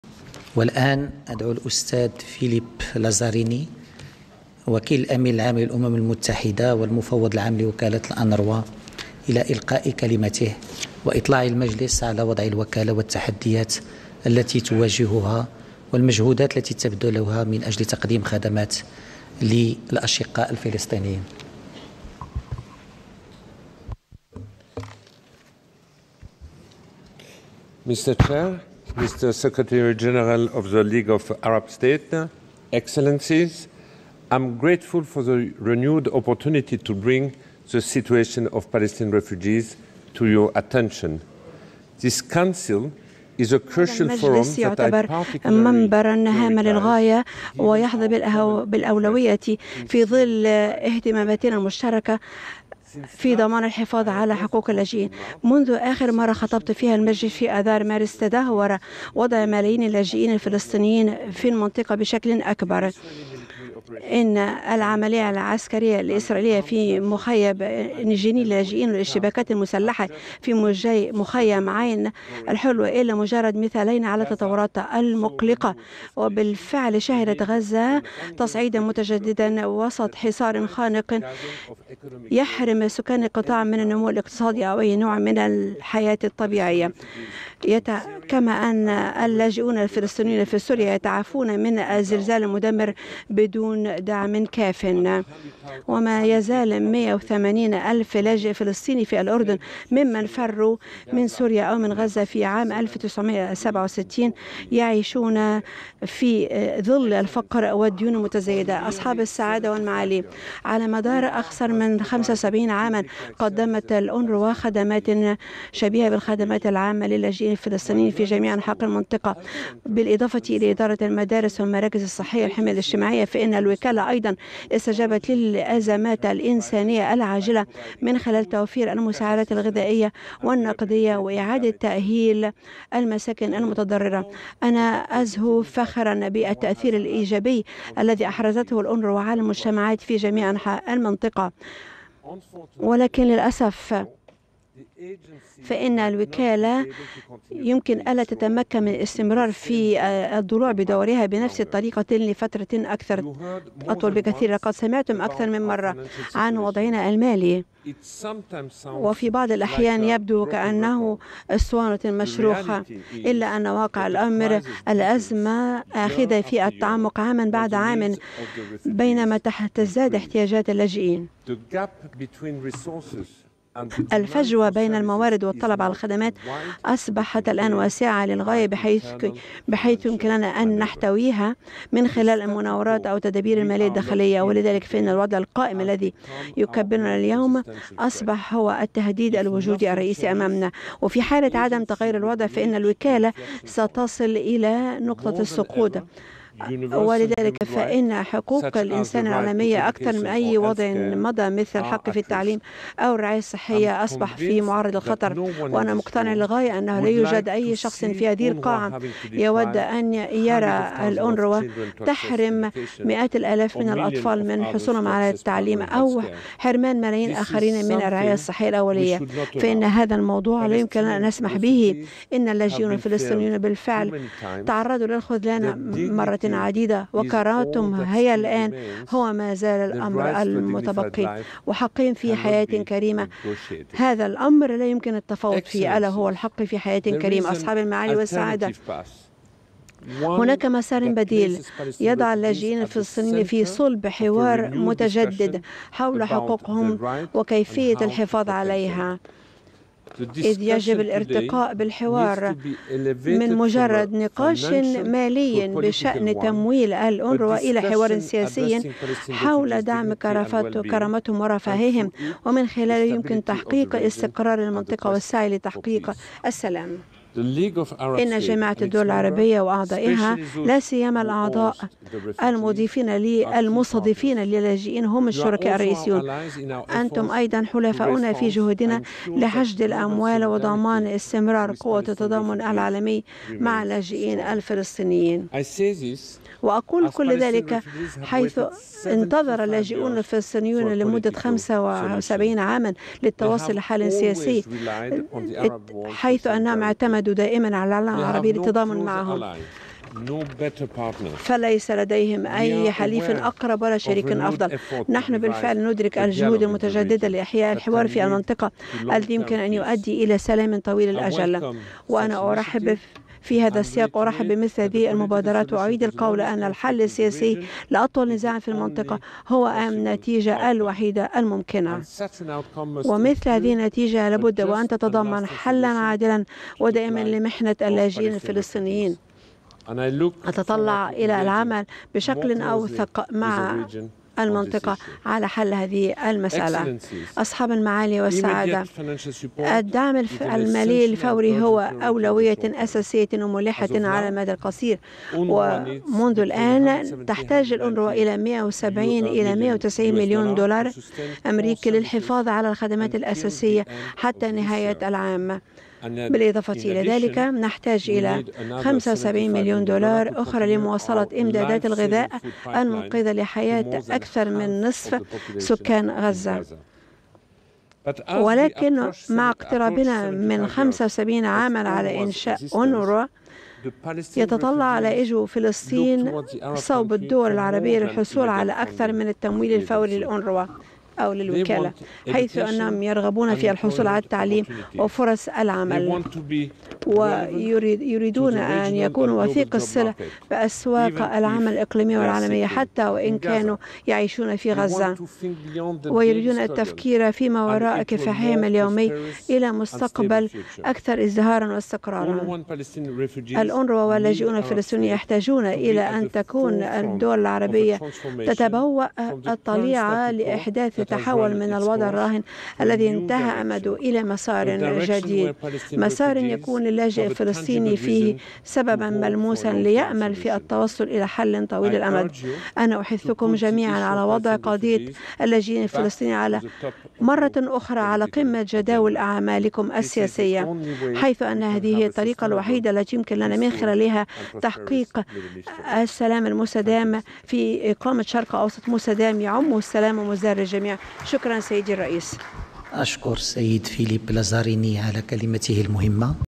كلمة فيليب لازاريني وكيل الأمين العام للأمم المتحدة خلال فعاليات الدورة 160لمجلس جامعة الدول العربية
كلمة فيليب لازاريني وكيل الأمين العام للأمم المتحدة خلال فعاليات الدورة 160 لمجلس جامعة الدول العربية على المستوى الوزاري

6/9/2023